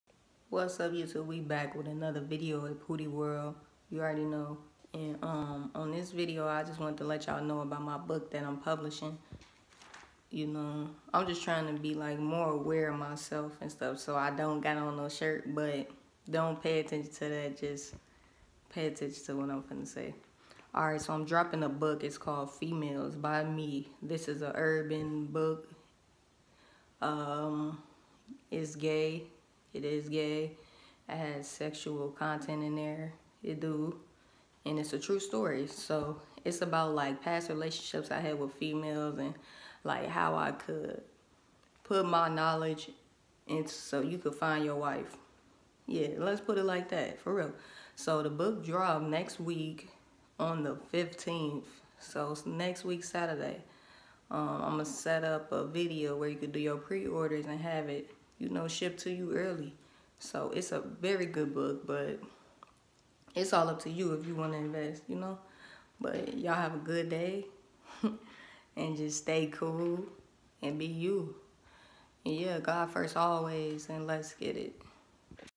January 15th Paperback Book
January 15th 2022 Females By: Y.G Paperback Book

On January 15th, I will be publishing my first book called “ FEMALES “ By Y.G

This book is a non fiction romance book about relationships with females as a stud individual

Great amazing book 📚
I recommend you sign up for pre-orders before the day January 15th

This is going to be epic ⭐️

Visa Gift after Sign up 🤽🏽‍♂️

⬆️⬆️⬆️ sign up just by entering your email + information ℹ️

DONT FORGET GOD FIRST 💯